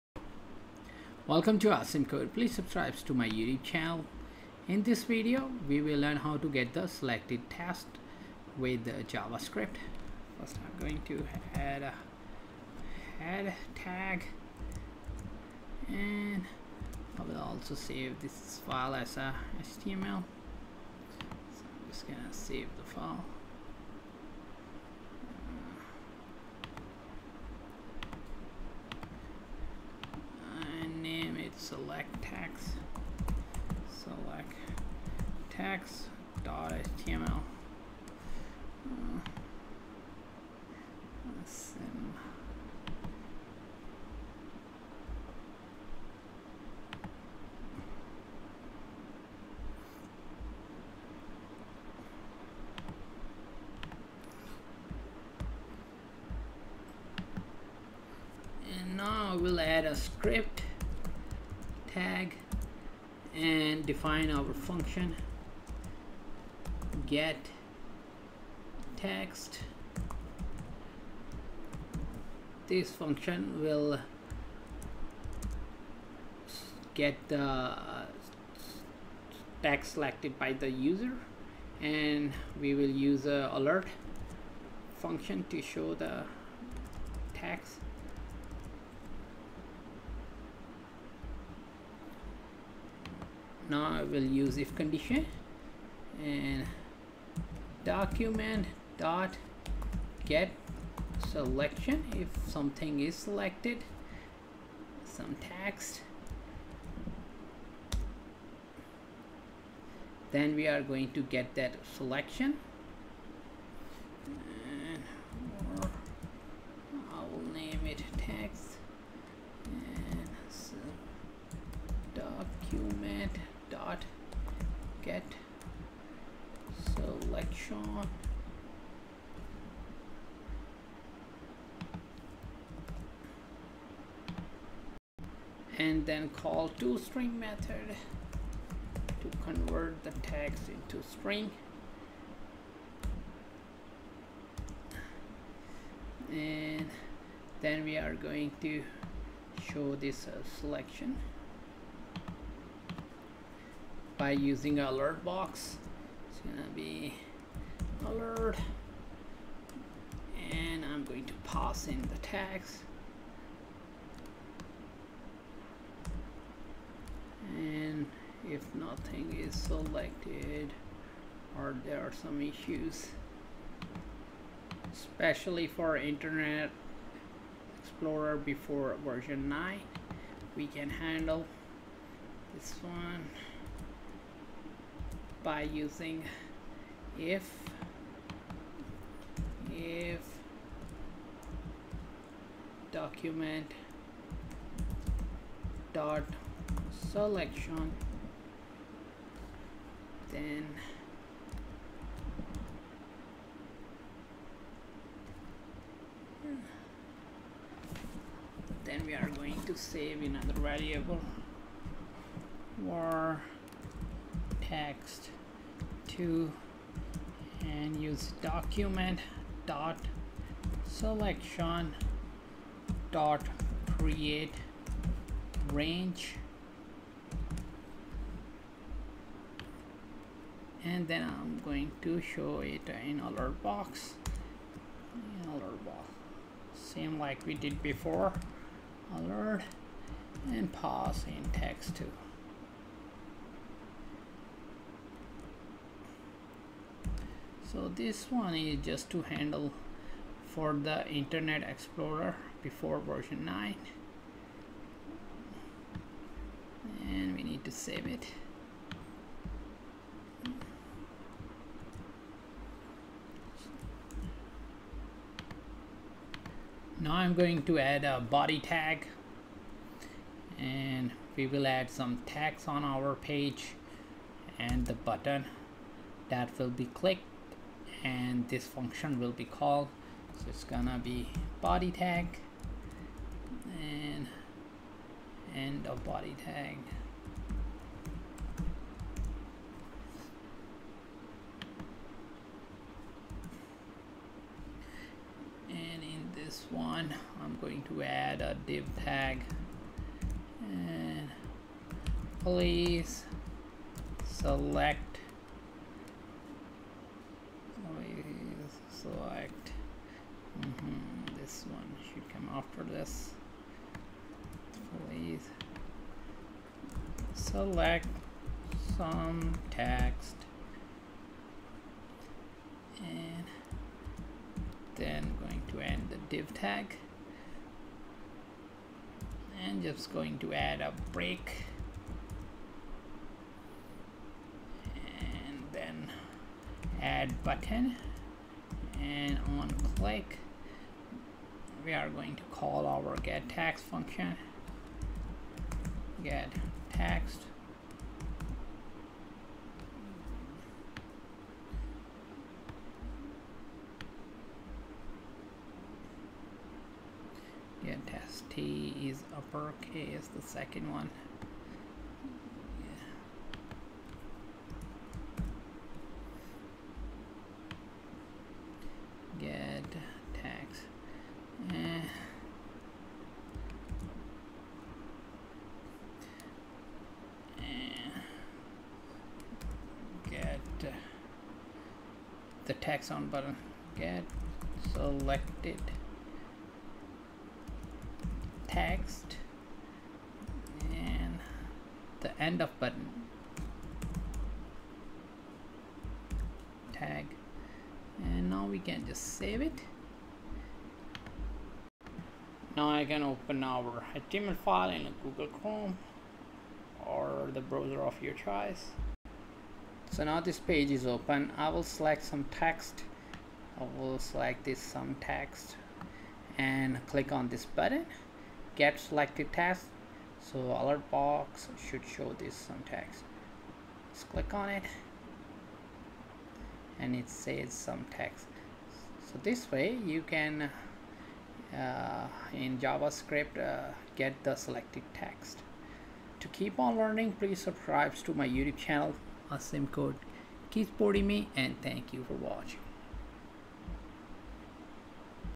How to get the selected text in JavaScript
In this video we will learn how to get the selected text on a Page in JavaScript.